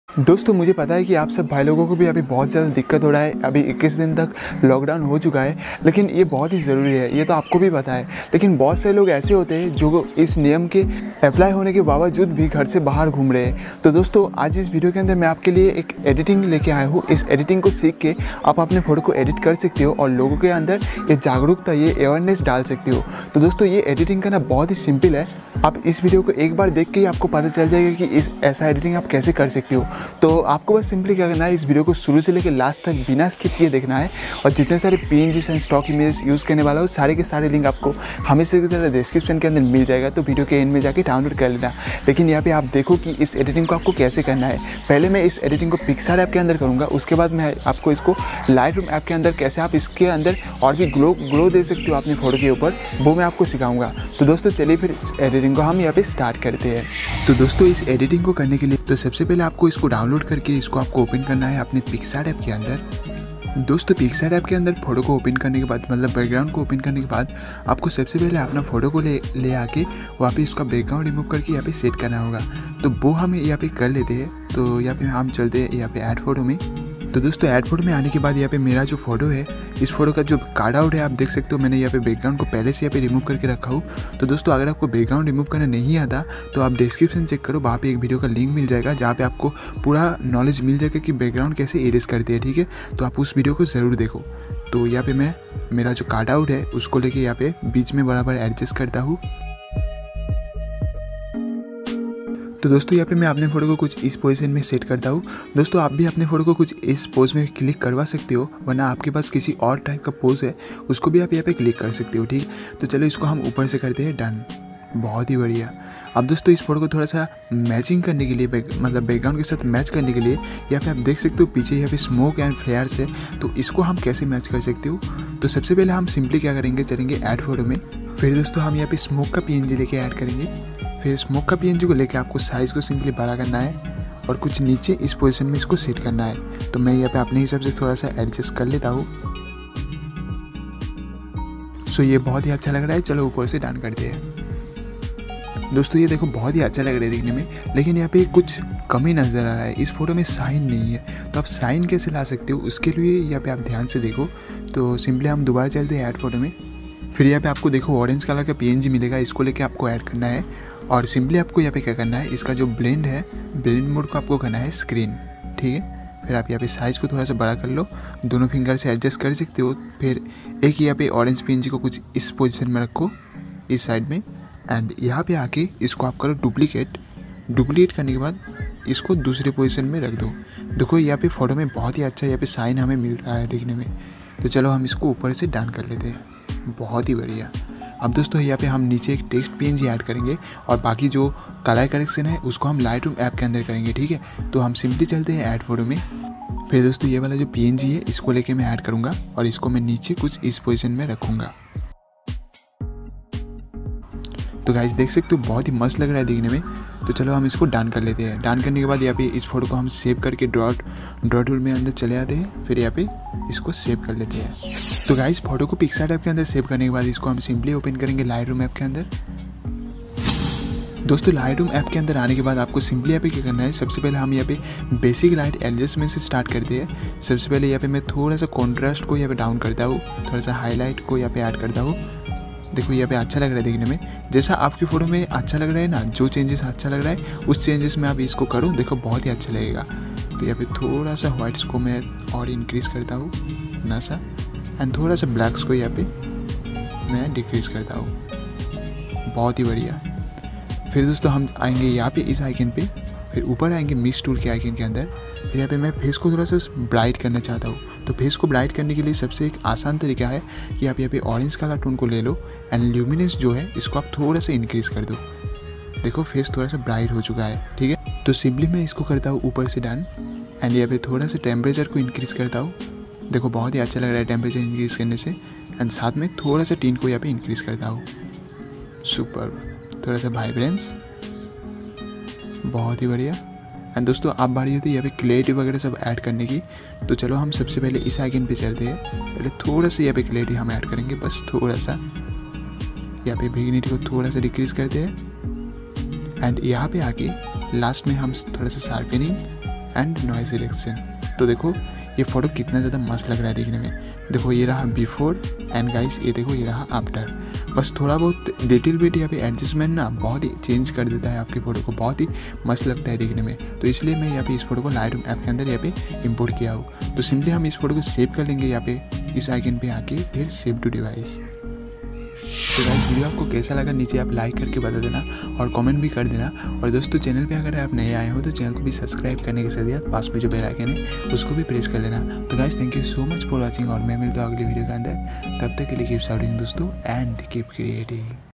Lockdown Photo Editing Concept in PicsArt 🔥|| Stay Home Save Lives || AC EDITION
In today's video I'll show you about an amazing photo editing concept just for sharing the quote of Stay Home, Save Lives.
First of all open the background in picsart and add your model image. Then to look the photo more realistic just add a smoke png. Afterthat add a orange color png and add Text png. Then save the photo and open it in Lightroom afterthat do adjustments as I show you in the Tutorial.

Guys follow the rules and Stay Home, Save Lives...